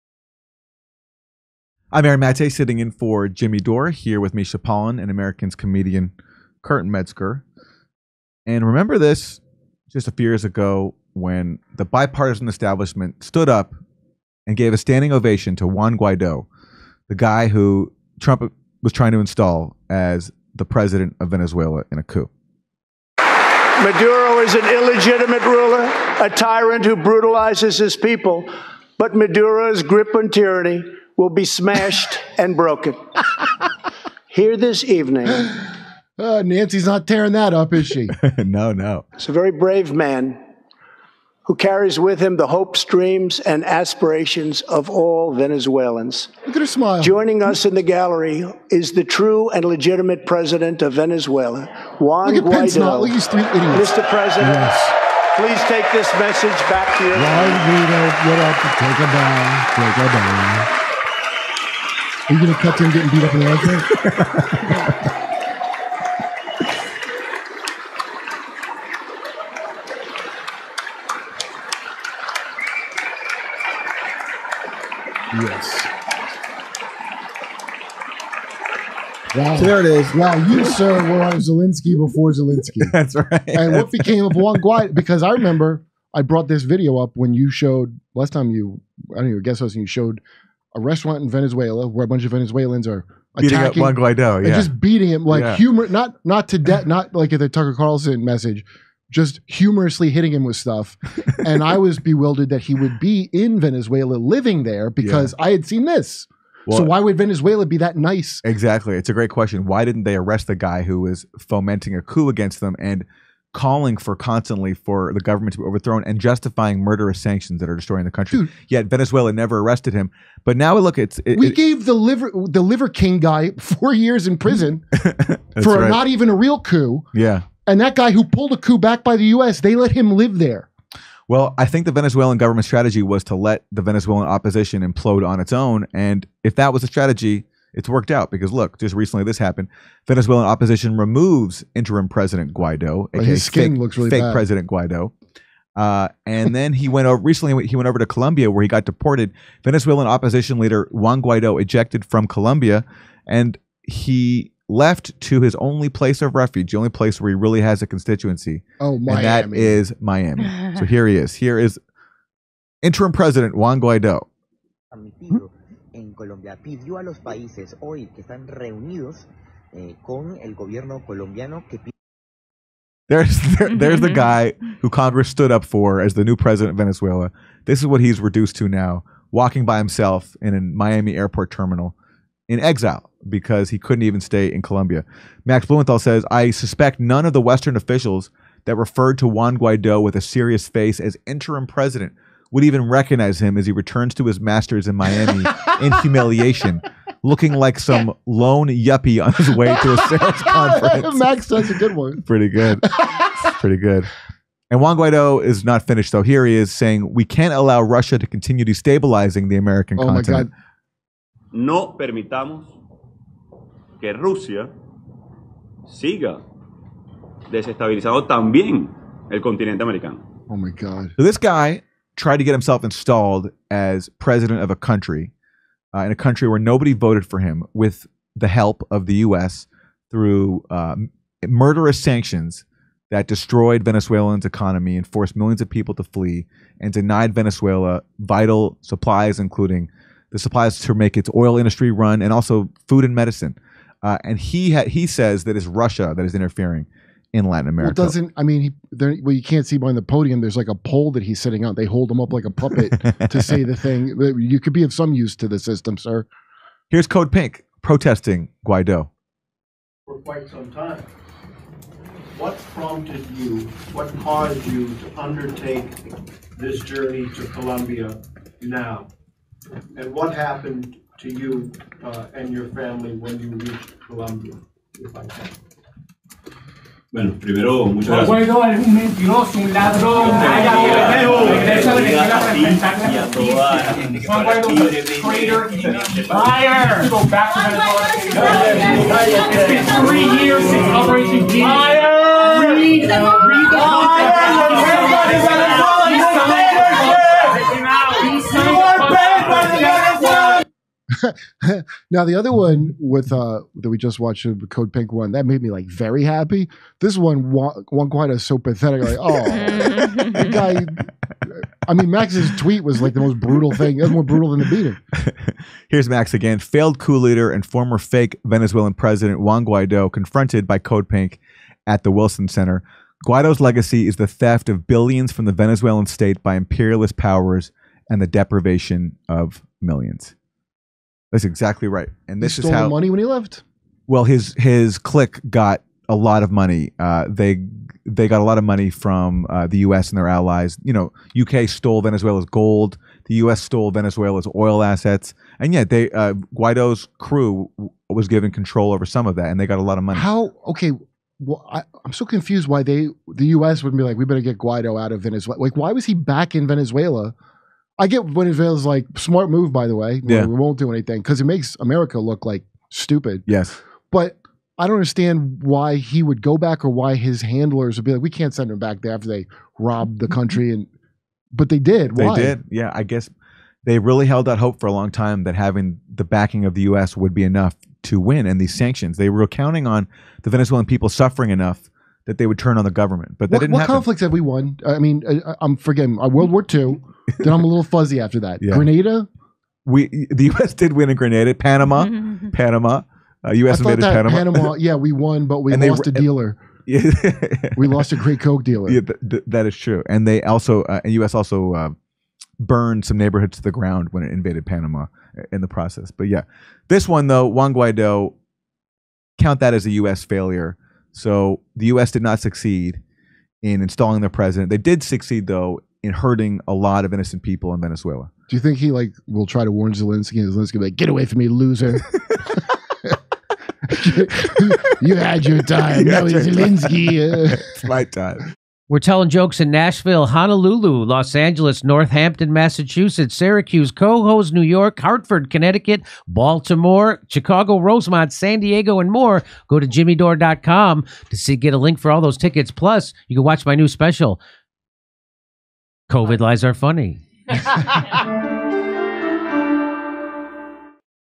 Fake Venezuelan President Juan Guaidó HUMILIATED
Venezuelan opposition leader and pretend President Juan Guaidó has left his home country, likely permanently, and arrived, like so many other failed Latin American puppets before him, in Miami. Guaidó was filmed doing the walk of shame as he disembarked, all alone, at Miami International Airport.

Guest host Aaron Maté and Americans’ Comedian Kurt Metzger discuss a recent event where CODEPINK’s Medea Benjamin disrupted Guaidó’s efforts to continue pushing U.S. imperialism in Venezuela.